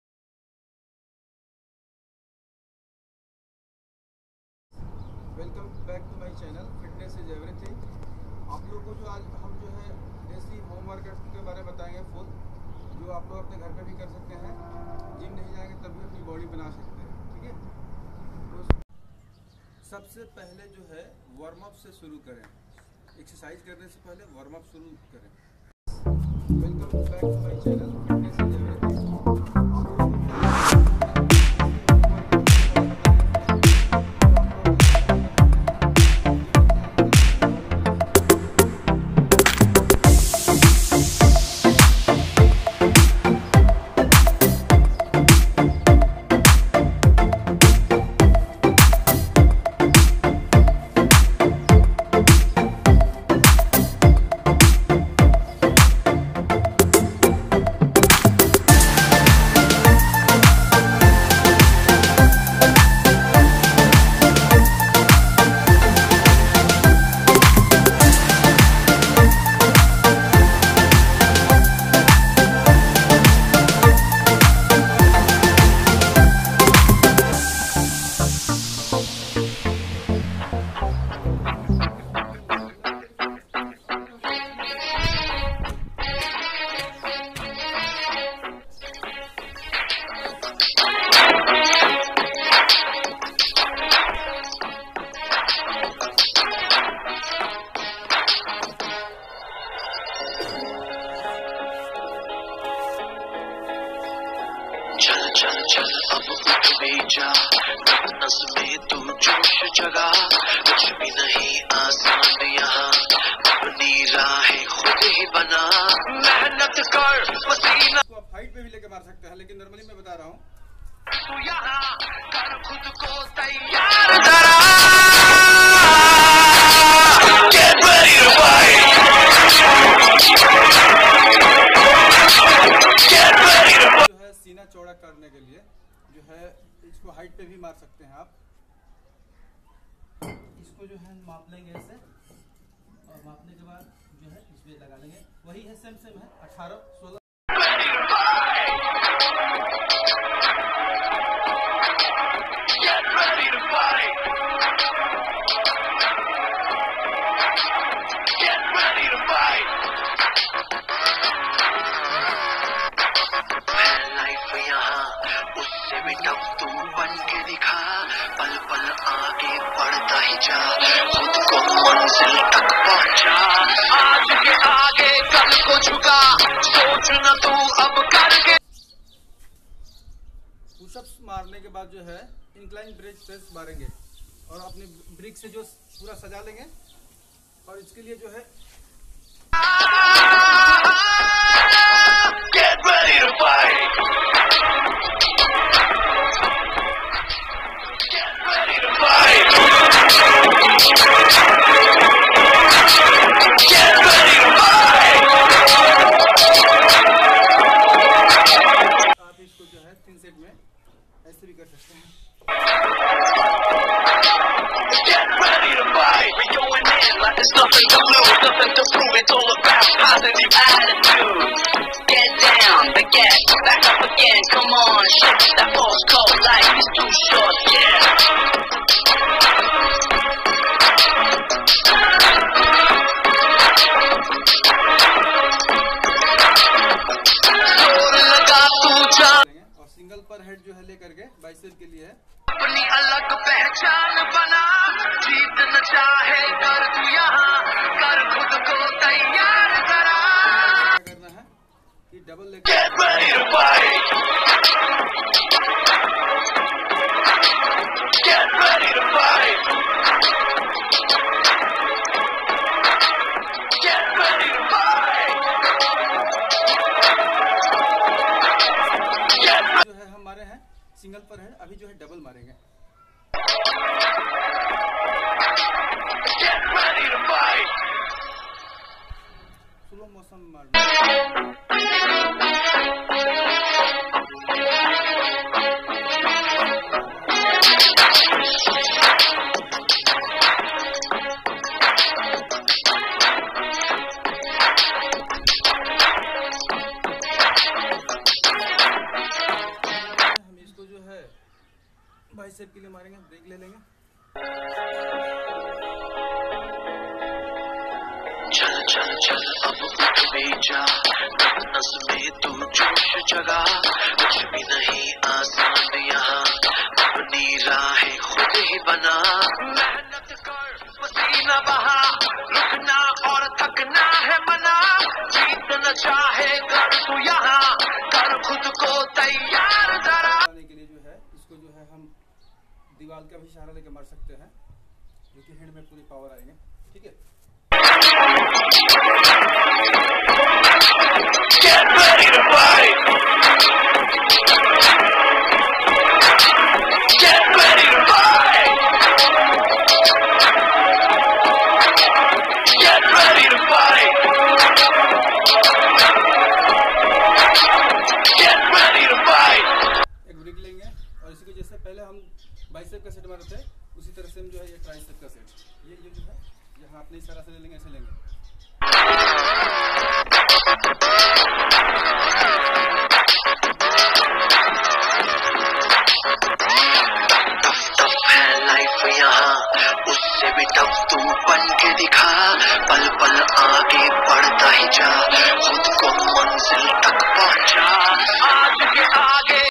Desi full workout
To make strong endurance ,stamina and good physique